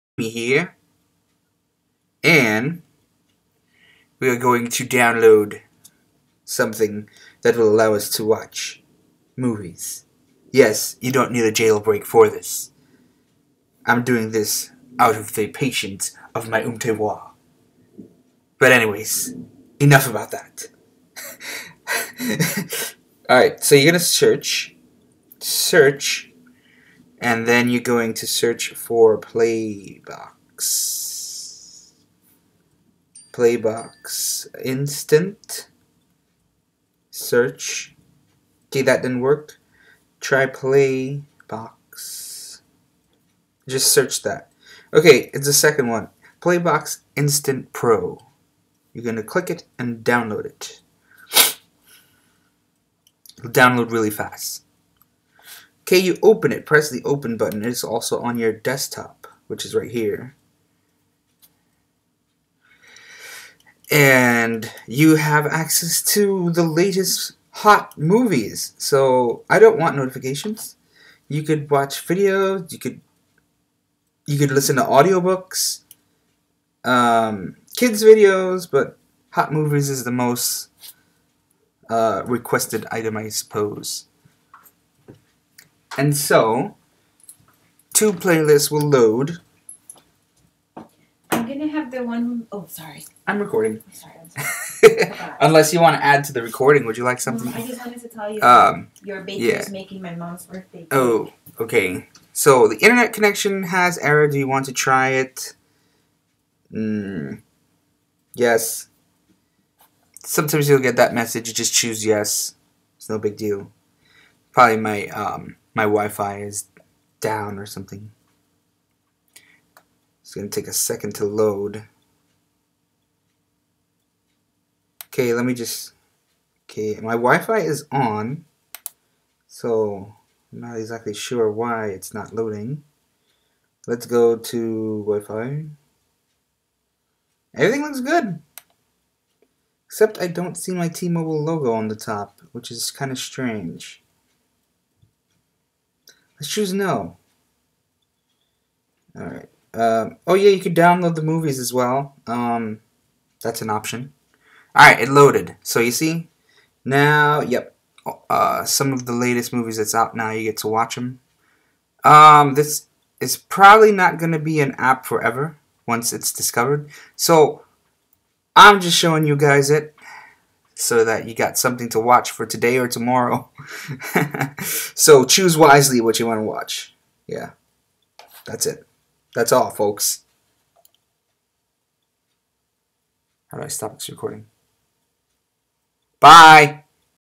How to watch New MOVIES??!! on iPhone
Here's a little app that i found that lets you watch some of the latest movies that are out on your iPhone or iPad etc.